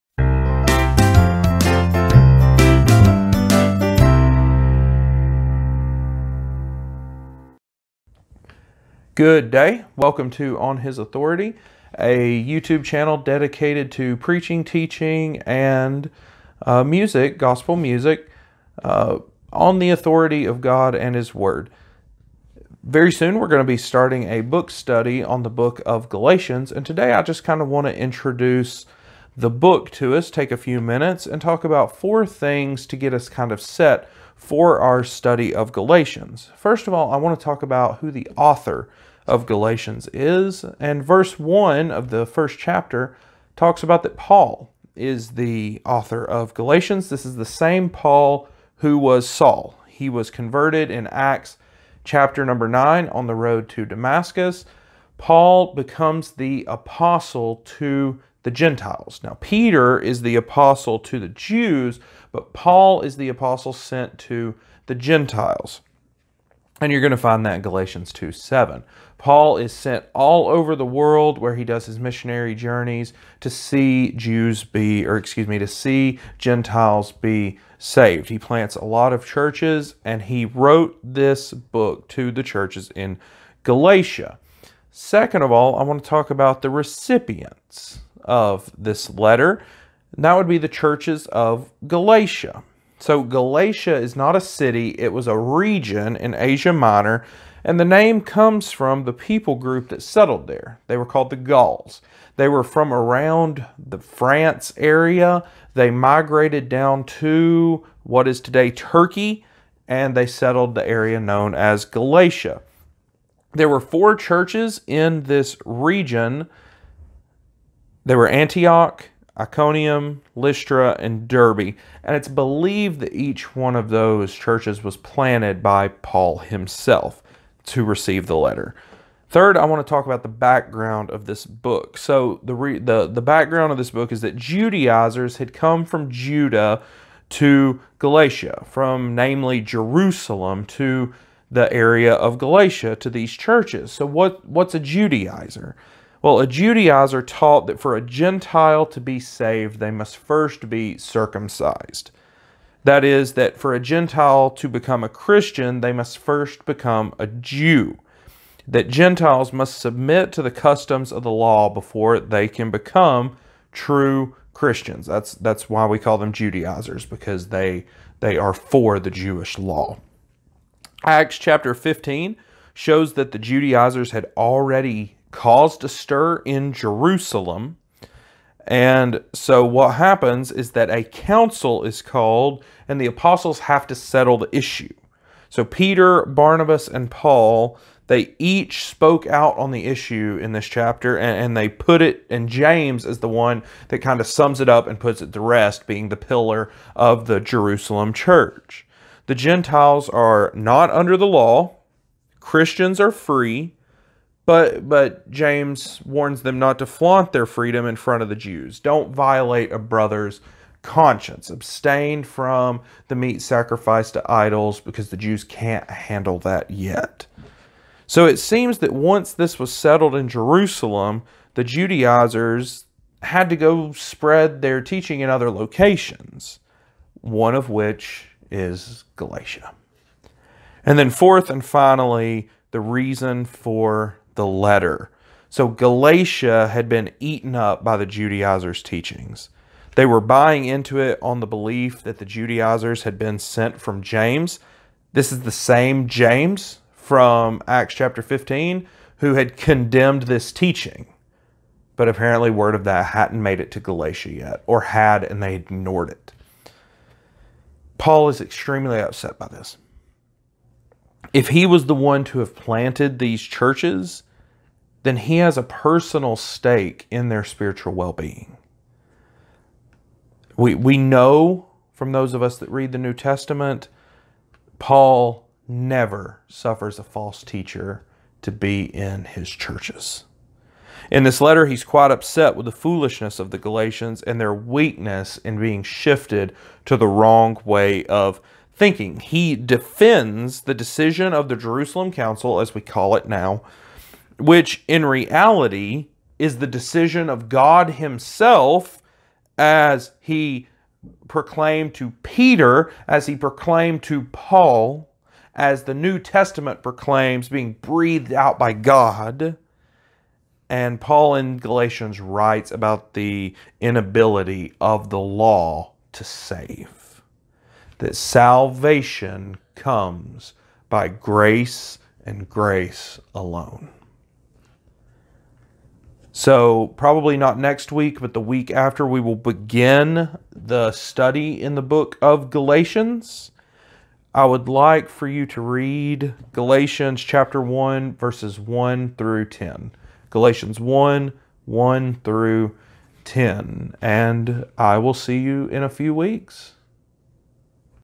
Introduction to Galatians
Welcome to On His Authority, a channel dedicated to gospel preaching, teaching, and music on the authority of God and his Word.

Today we will be starting a book study into Paul's letter to the Galatians.
Reading for next week will be Galatians 1:1-10.